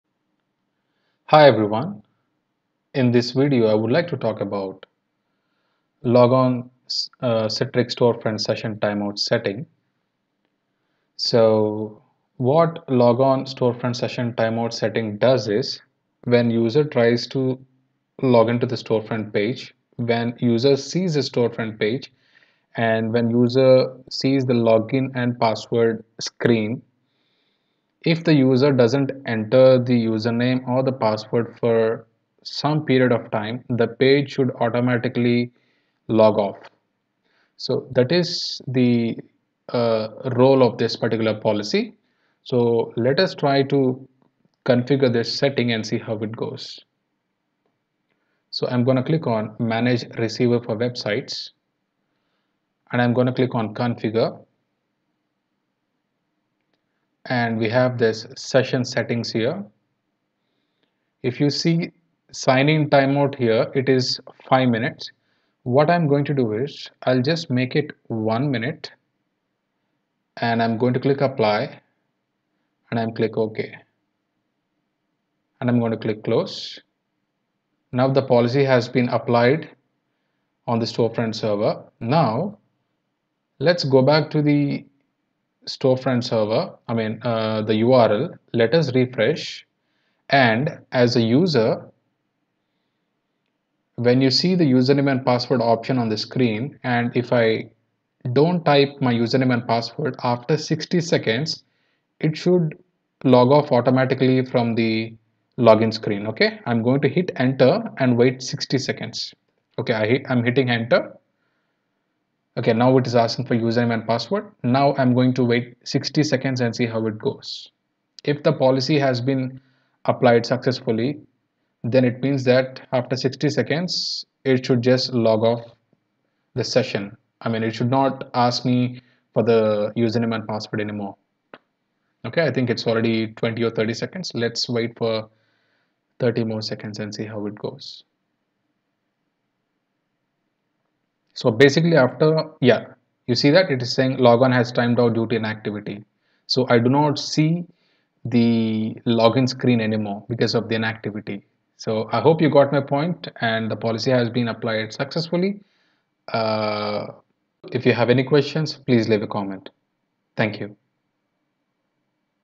Testing Citrix Storefront login screen session timeout Policy
In this video, I have configured login session timeout when there is inactivity for 60 seconds, policy has been applied successfully as we have tested it and noticed the same as a end user.